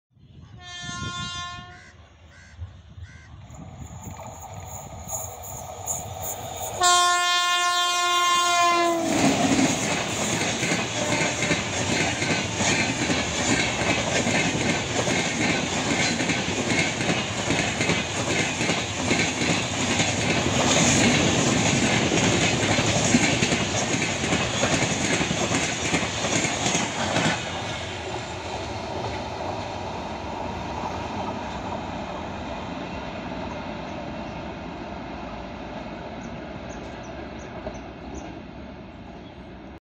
King returns home after visiting Chennai. (22675) Cholan SF Express.
WAP 7 (Loco shed: Royapuram) hauling Cholan SF Express.

Spotted at Thiruverumbur.

It runs via Villupuram, Cuddalore Port Jn, Chidambaram, Sirkazhi, Vaithiswaran Kovil, Mayiladudhurai, Kumbakonam, Papanasam and Thanjavur.